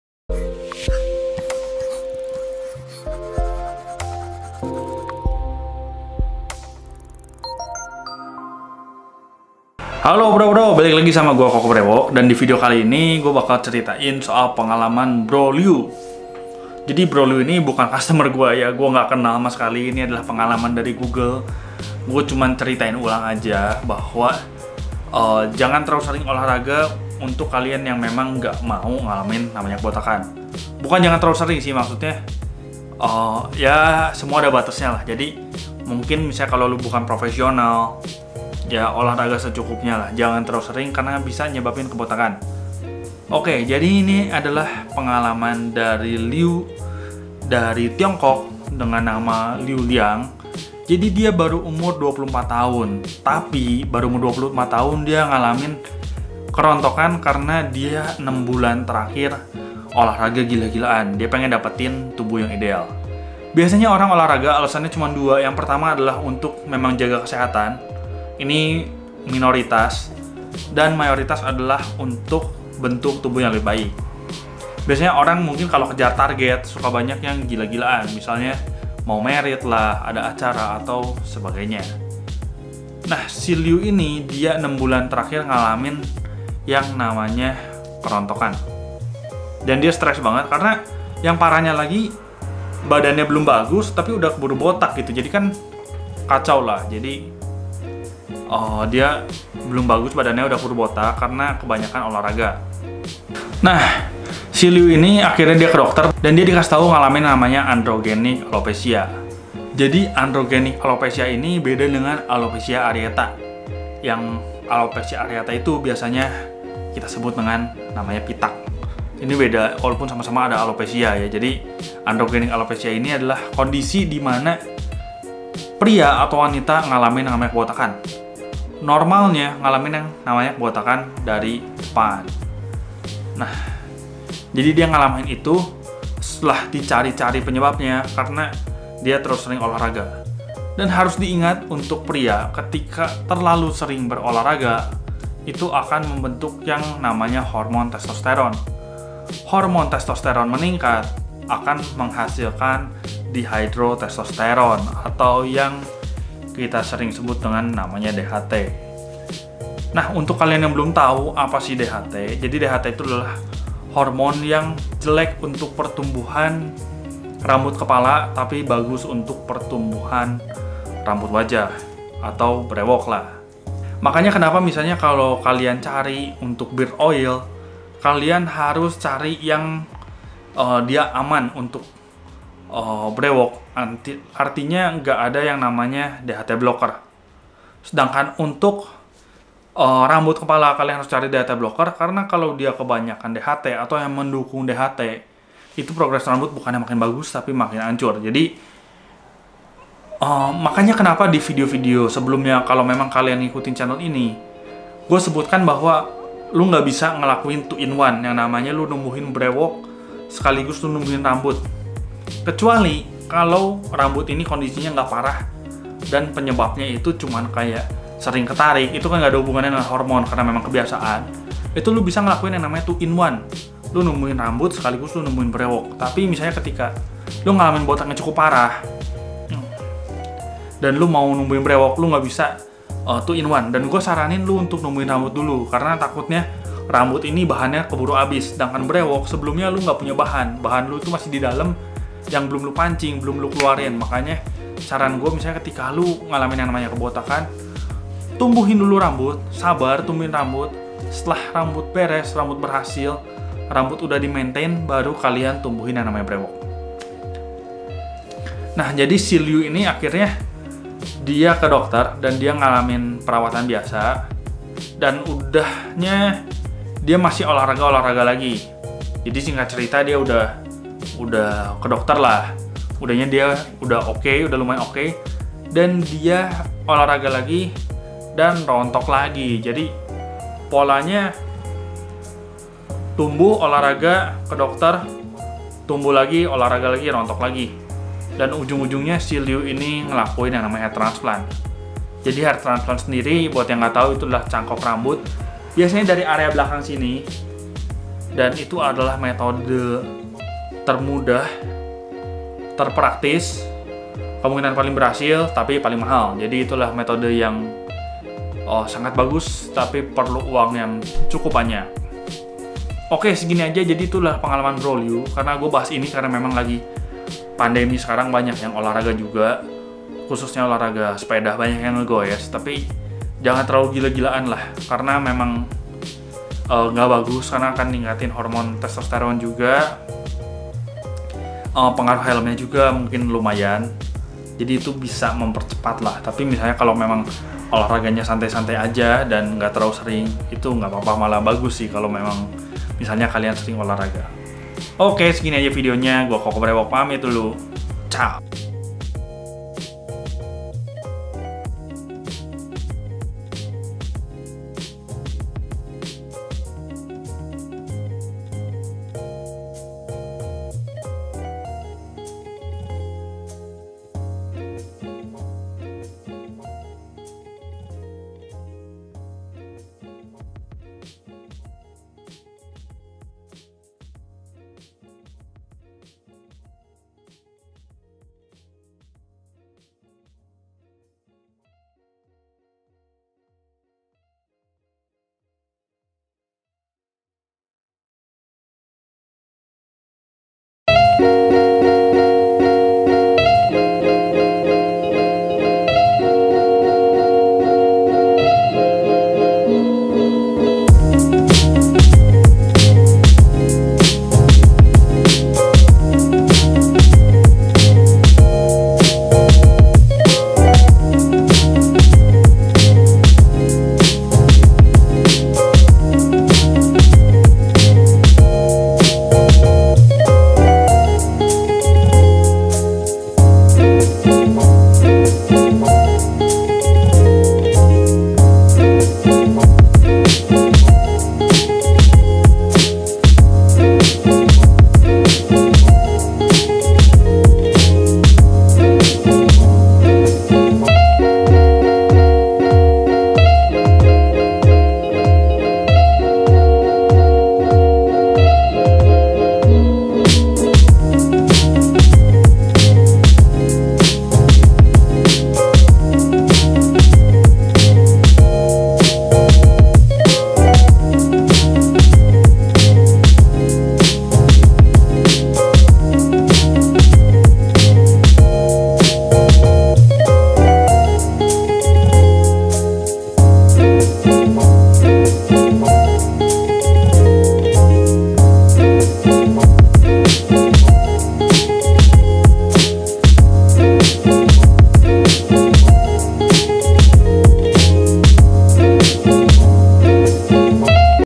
HATI HATI !!! TERLALU SERING OLAHRAGA BISA SEBABKAN KEBOTAKAN !!!
ati ati bro, sehat kaga, malah jadi botak